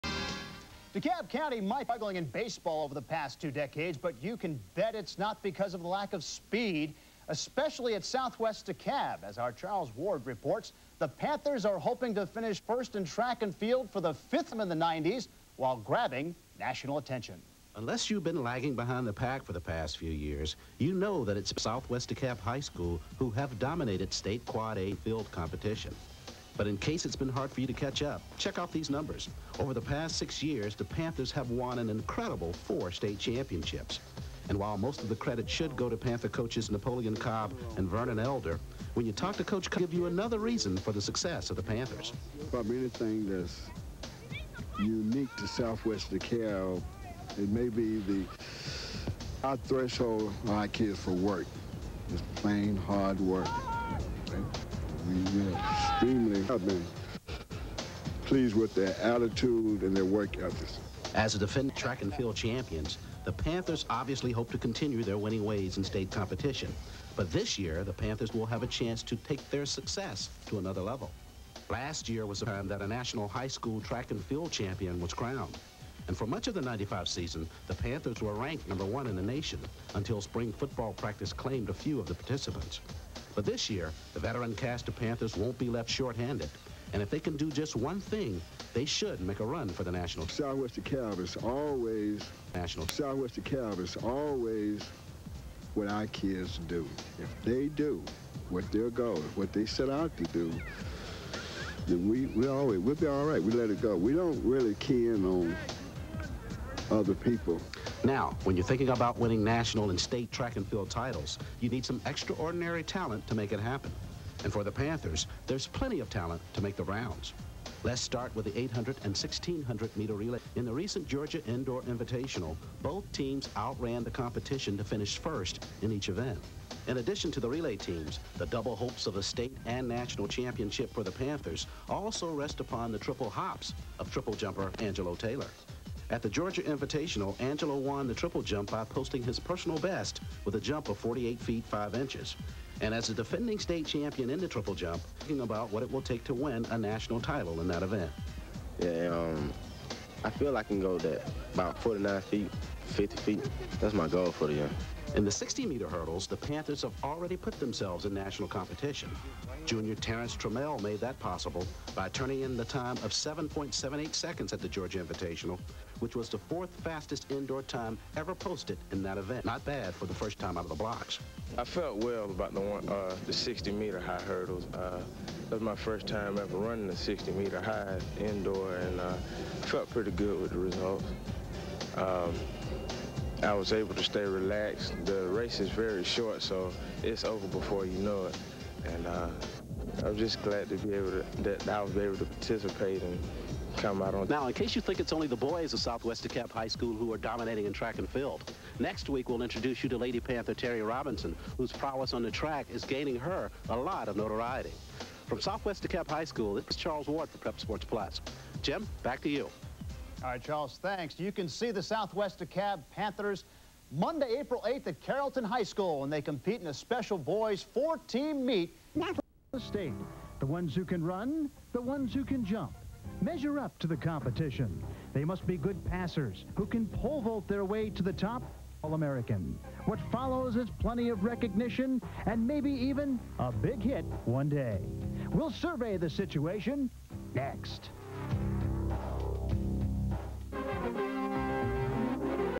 PrepSportsSWDTrack96
THIS IS A VIDEO ABOUT SOUTHWEST DEKALB HIGH SCHOOL 1996 NATIONAL CHAMPIONSHIP TRACK TEAM, LEAD BY HEAD COACH COBB AND ASSISTANT COACH ELDER. THIS TEAM ALSO FEATURED TO FUTURE OLYMPIANS ANGELO TAYLOR AND TERRENCE TRAMMEL.